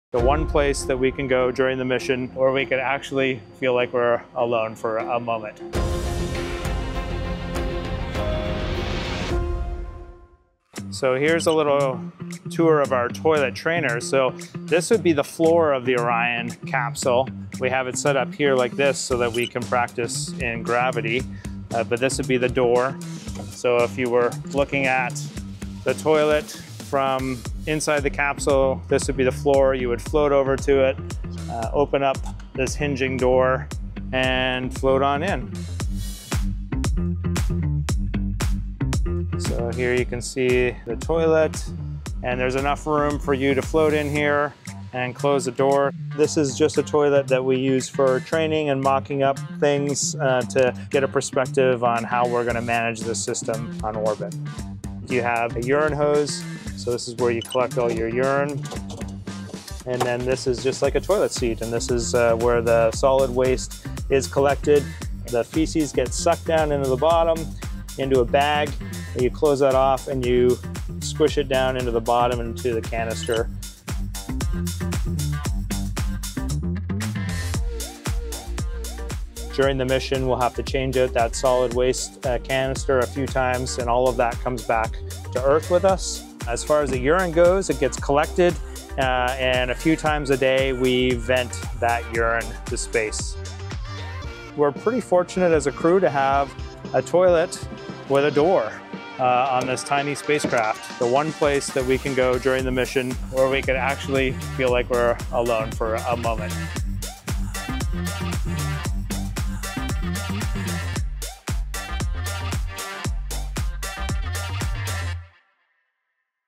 Vlog 18: The lunar loo – or going to the bathroom during a mission to the Moon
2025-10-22 – In this vlog, CSA astronaut Jeremy Hansen takes a deep dive into one of the questions that astronauts are most frequently asked: how they go to the bathroom in space. Take a tour of Orion’s toilet to learn about how business is done, waste is managed and privacy is ensured in a campervan-size space capsule with four astronauts.

Credits: Canadian Space Agency, NASA, Fulwell 73 UK Limited